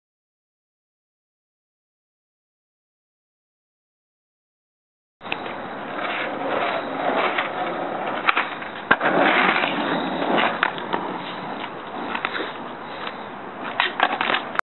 Grand Blanc High school drop
Joseph ollieing the highschool drop.